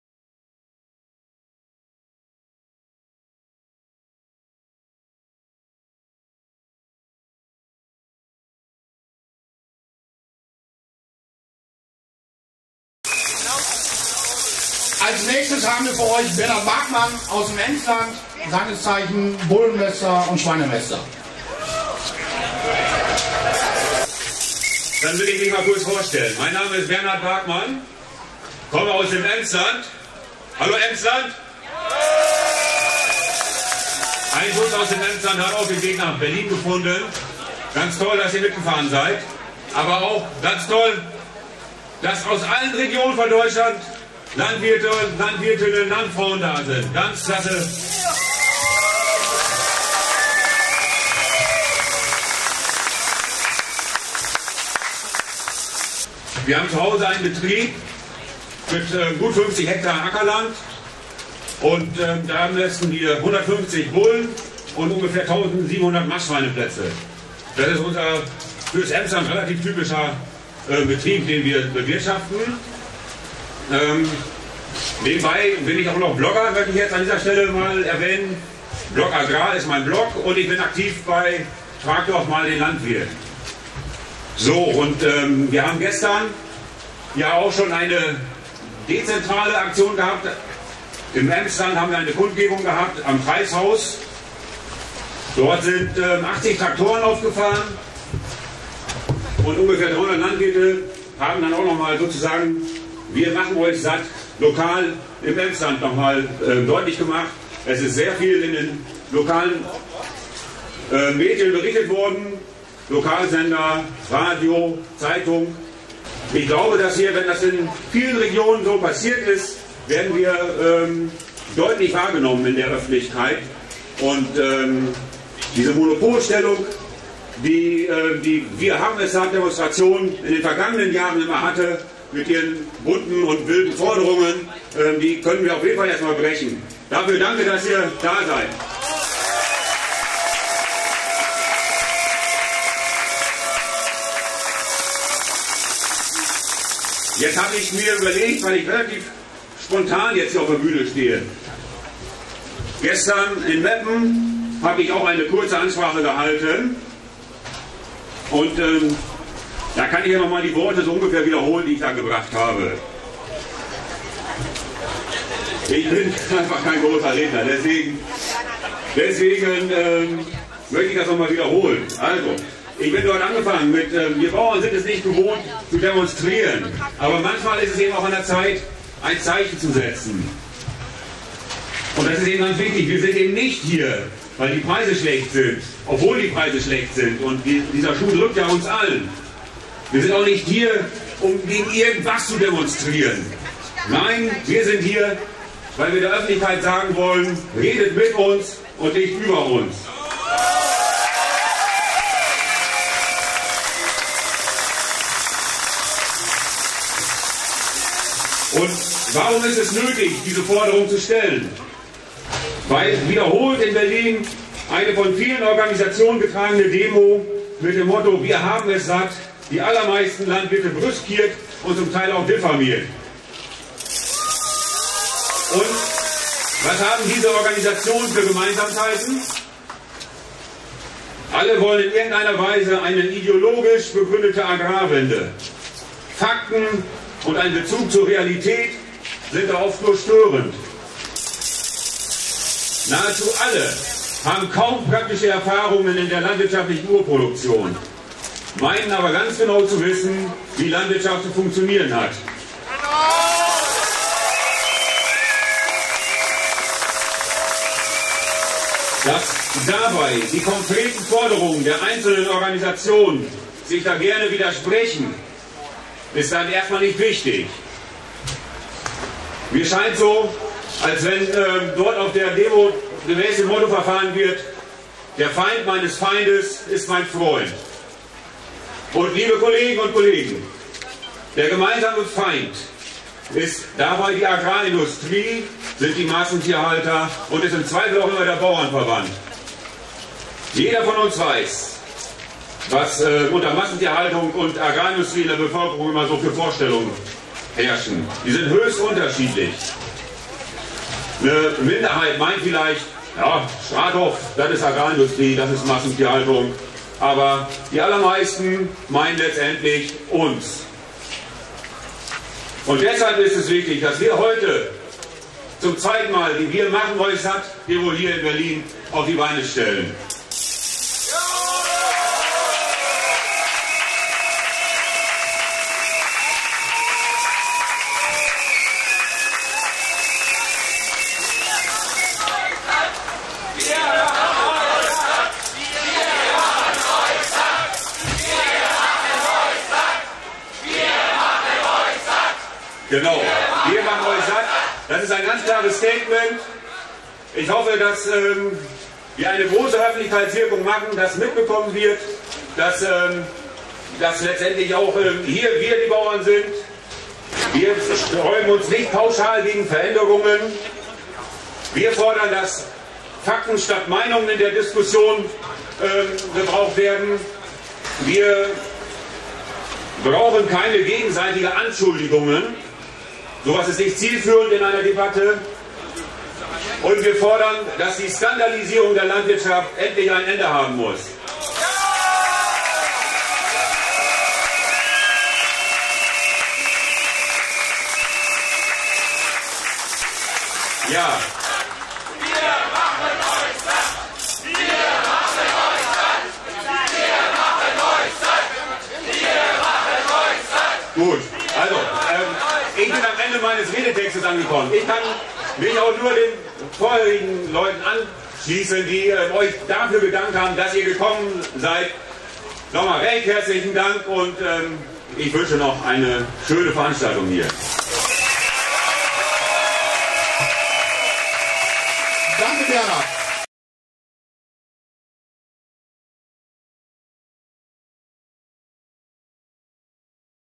Wir machen euch satt 2.0: Bernhard Barkmann
Schweine- und Bullenmäster aus dem Emsland